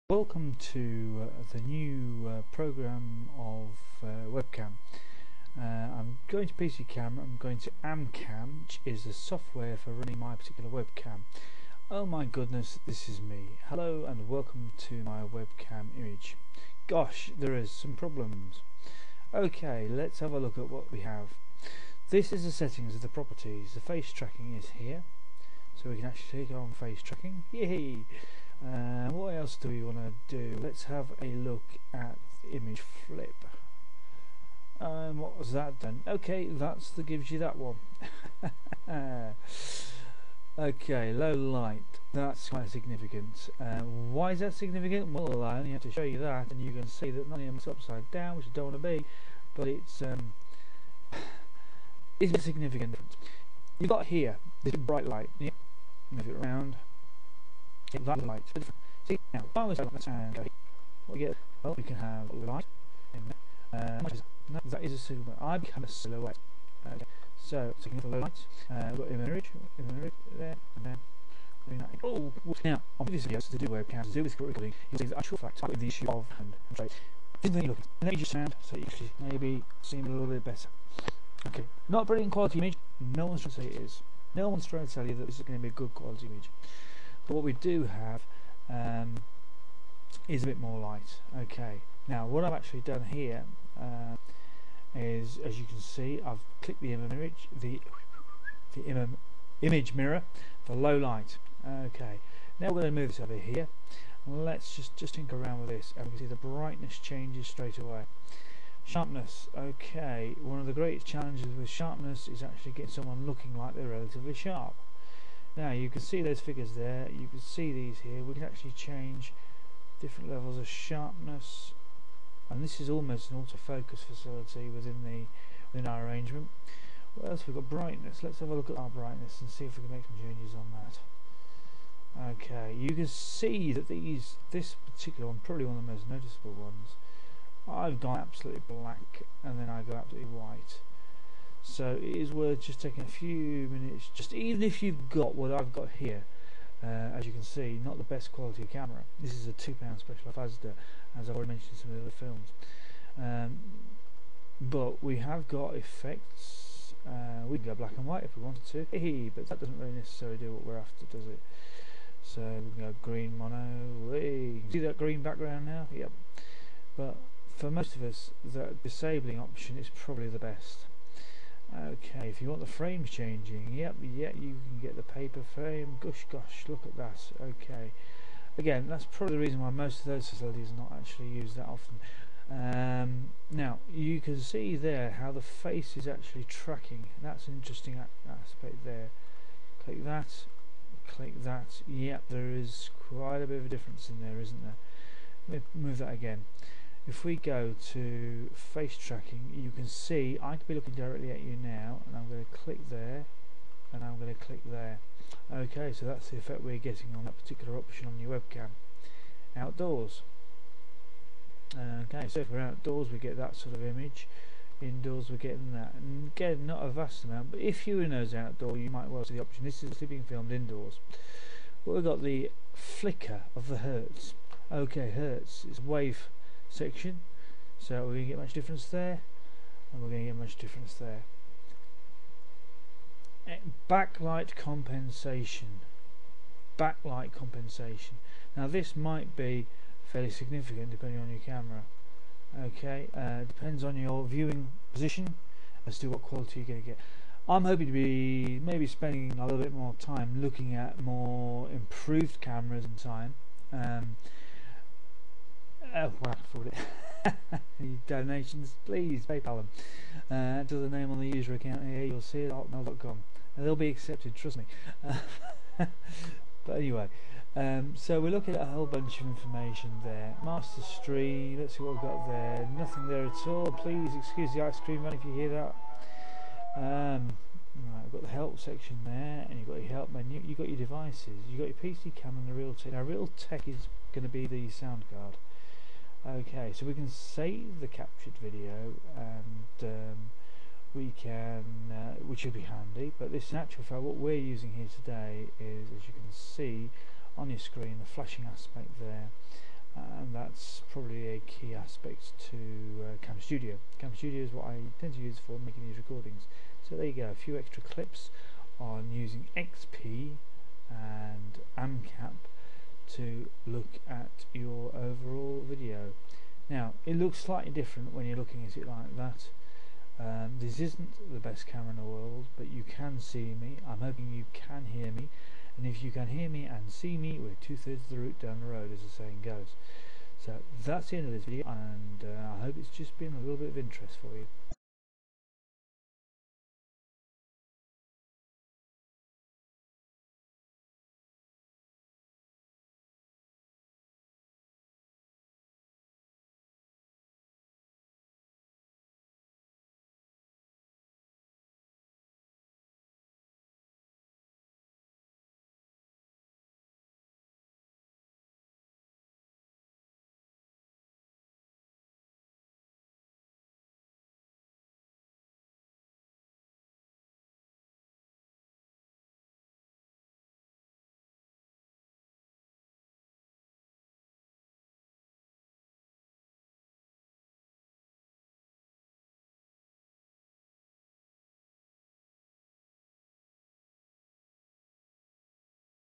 A web cam and its settings and how to change them
how to change a web cam settings using aMcam